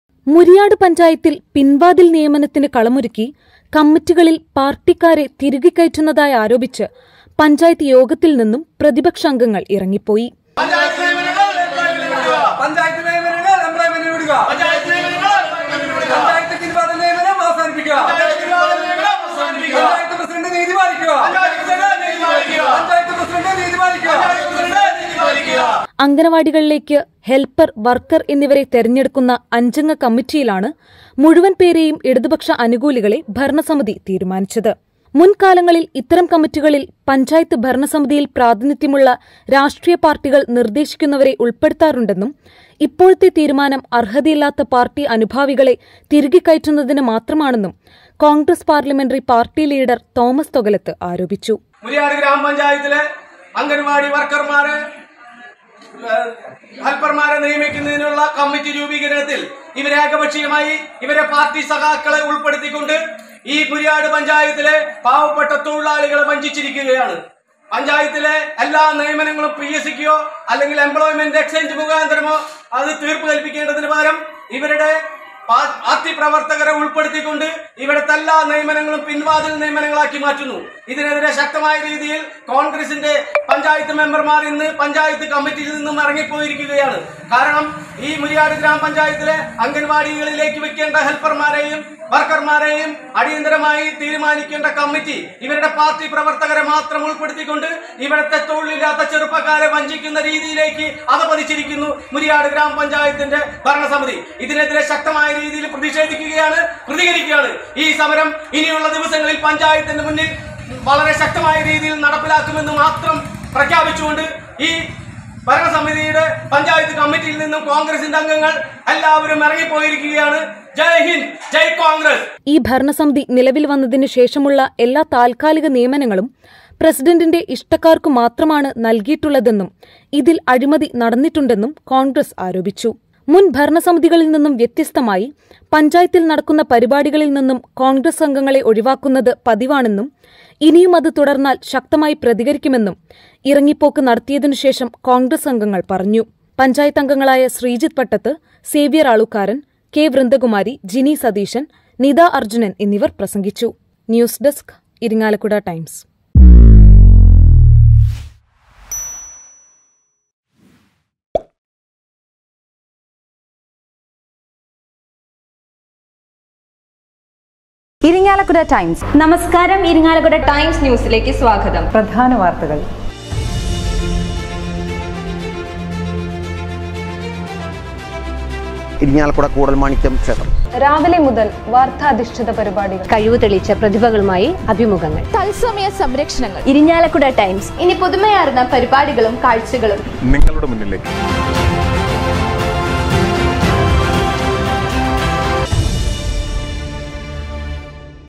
മുരിയാട് പഞ്ചായത്തിൽ പിൻവാതിൽ നിയമനം : കോൺഗ്രസ്സ് അംഗങ്ങൾ ഇറങ്ങിപ്പോയി...
മുരിയാട് പഞ്ചായത്തിൽ പിൻവാതിൽ നിയമനം നടക്കുന്നതായി ആരോപണം : പഞ്ചായത്ത് യോഗത്തിൽ നിന്ന് കോൺഗ്രസ്സ് അംഗങ്ങൾ ഇറങ്ങിപ്പോയി

മുരിയാട് പഞ്ചായത്തിൽ പിൻവാതിൽ നിയമനത്തിന് കളമൊരുക്കി കമ്മിറ്റികളിൽ പാർട്ടിക്കാരെ തിരുകി കയറ്റുന്നതായി ആരോപിച്ച് പഞ്ചായത്ത് യോഗത്തിൽ നിന്നും പ്രതിപക്ഷ അംഗങ്ങൾ ഇറങ്ങിപ്പോയി.